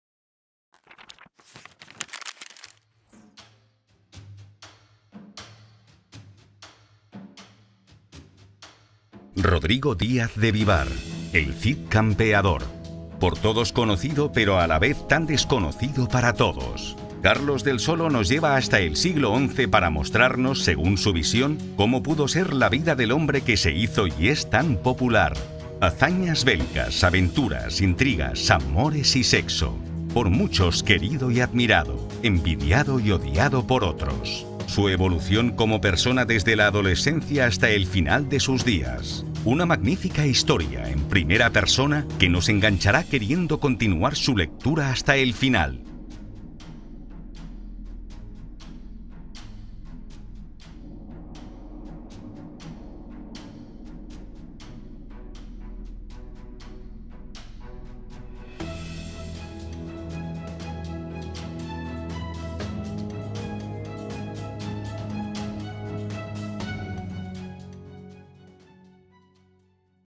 Trailer de la novela El Cid Campeador. Simplemente Rodrigo
Tráiler "de cine" de la novela de Carlos del Solo "El Cid Campeador. Simplemente Rodrigo" realizada por la editorial Letrame